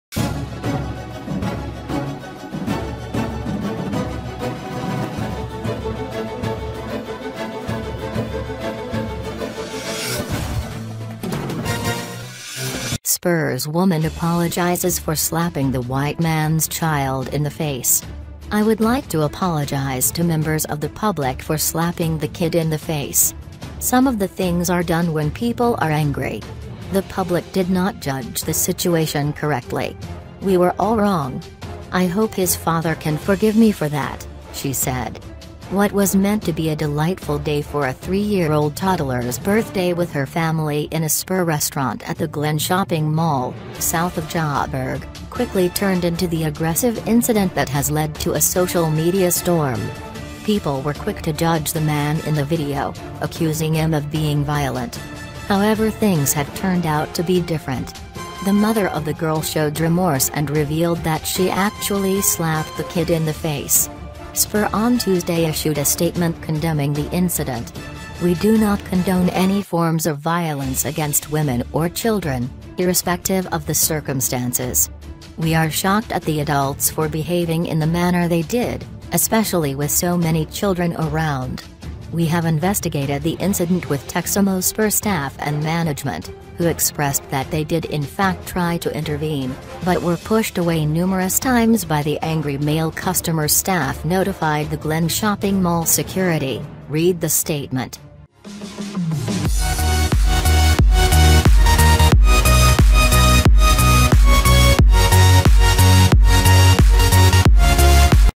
Spurs woman apologizes for slapping the white man’s child in the face
Spurs woman apologizes for slapping the white man’s child in the face. “I would like to apologize to members of the public for slapping the kid in the face. some of the things are done when people are angry. The public did not judge the situation correctly. We were all wrong. I hope his father can forgive me for that,” she said.

✔ Sesethu Zikhona’s 61 year old Sugar Daddy arrested as it turns out he is the boyfriend